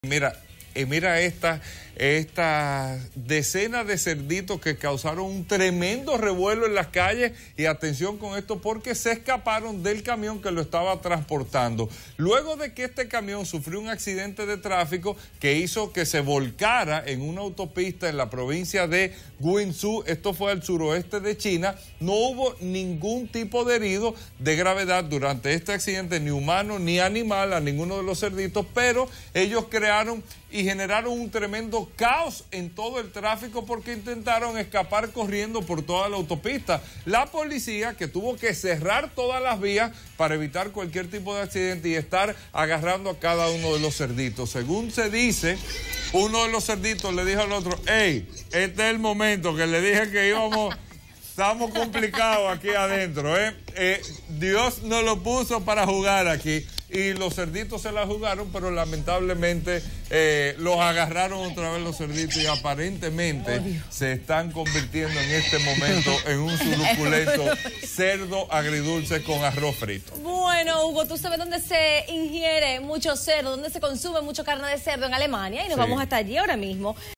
Cerditos sueltos en una carretera de China desatan el caos
En Más que Noticias: unos cerditos sueltos en una carretera de China causaron tremendo revuelo tras escaparse.